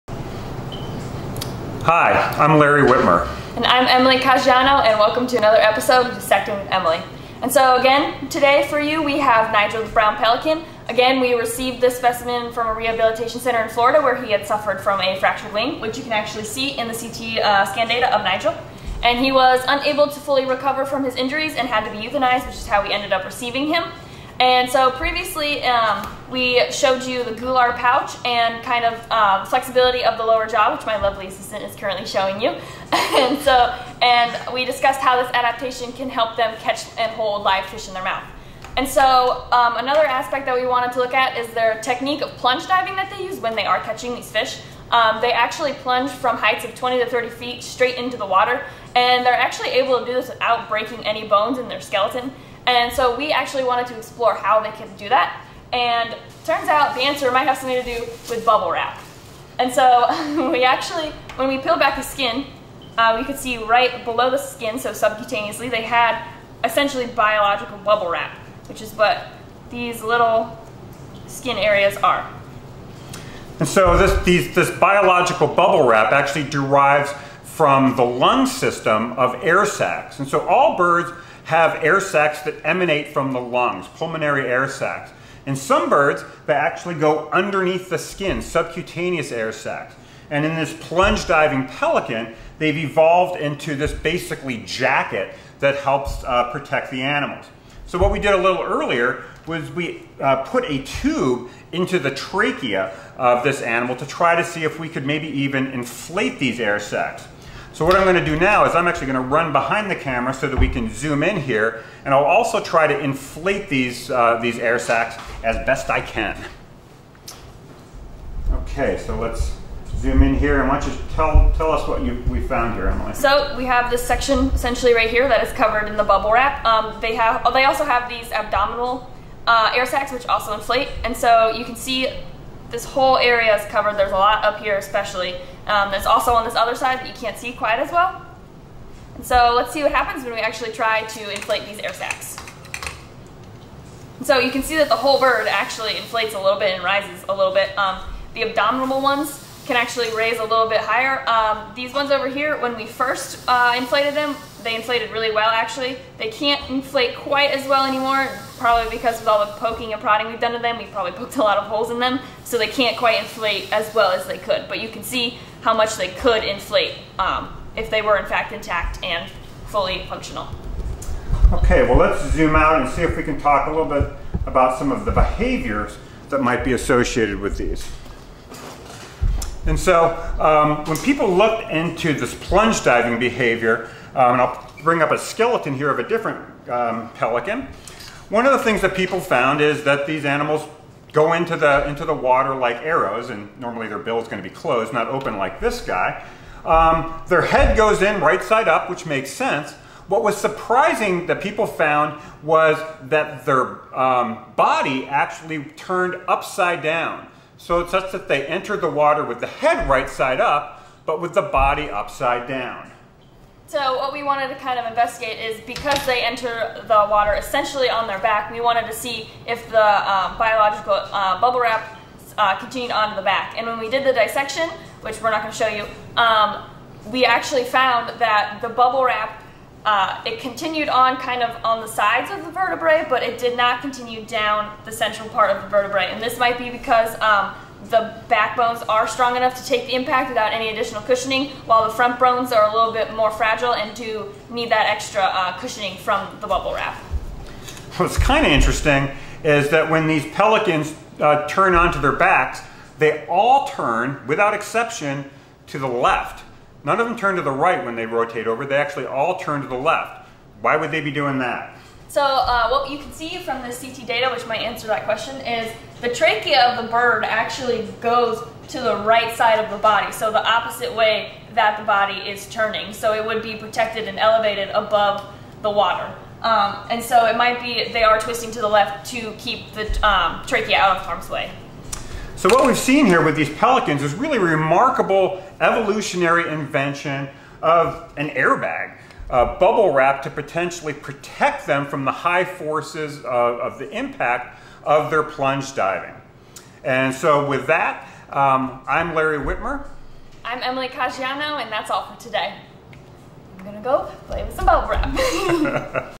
Dissecting with Emily - Brown pelican - biological bubble wrap
Another episode of "Dissecting with Emily"! This time, Ohio University Honors Tutorial College star sophomore Emily Caggiano and I are looking at the subcutaneous air sacs that form a layer of "biological bubble wrap" under the skin of brown pelicans. These help the birds survive plunge diving by forming something of an airbag to cushion the impact. We also discuss some of the anatomical peculiarities associated with how they rotate their bodies just before entry. This is the same poor pelican, whom we're calling Nigel in honor of Finding Nemo, who had to be euthanized at the wildlife rehab clinic in Florida. We talked about Nigel and his gular pouch in a previous episode of Dissecting with Emily Check out the whole Dissecting with Emily series with this playlist For other content like this, check out our WitmerLab Facebook